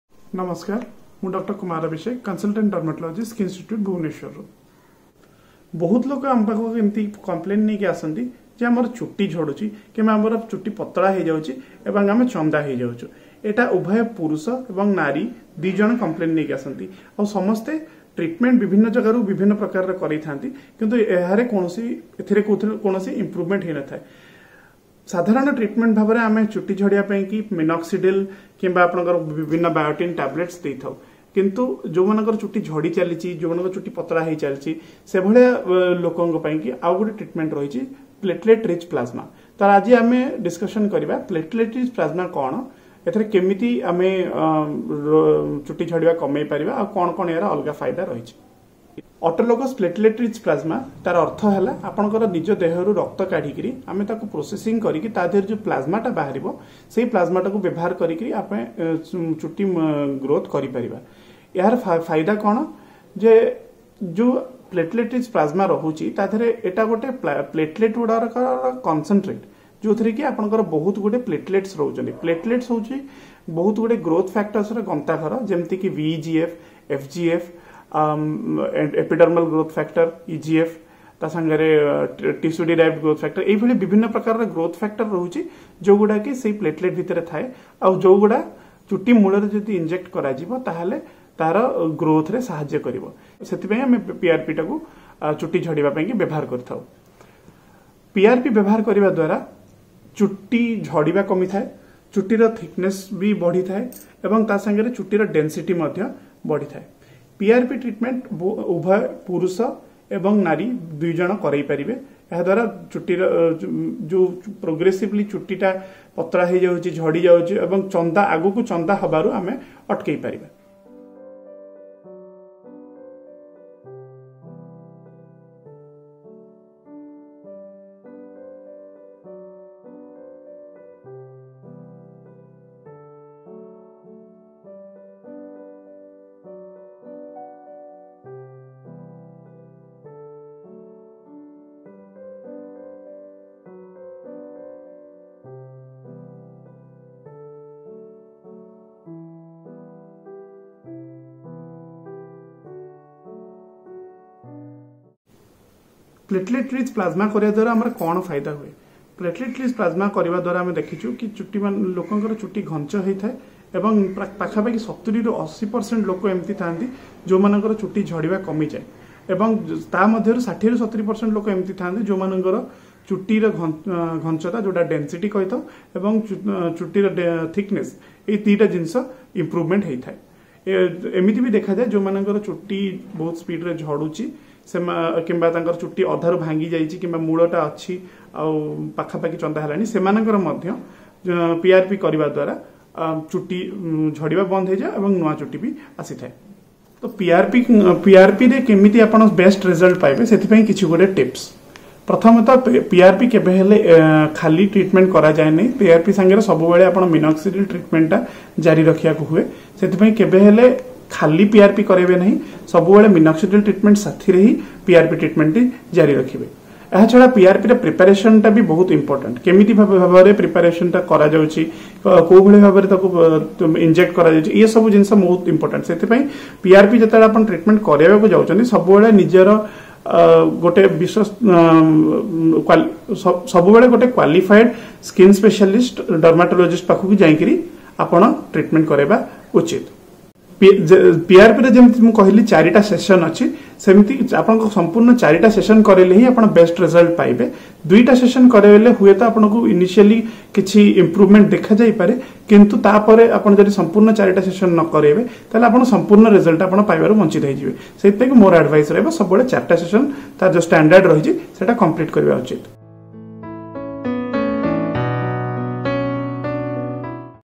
PLATELET RICH PLASMA FOR HAIRLOSS -(In Odia)
PLATELET RICH PLASMA contains a range of growth factors and proteins that speed tissue repair. Platelet rich plasma for hairloss is an attractive option. As some types of hair loss result from damage to hair follicles, researchers initially hypothesized that PRP could help regrow hair by reversing the process that occurs in androgenetic alopecia.

Since then, PRP has become a popular method of restoring hair growth. Doctors have also used PRP to treat injuries to the tendons, muscles, and ligaments, such as those that people sustain during sporting activities.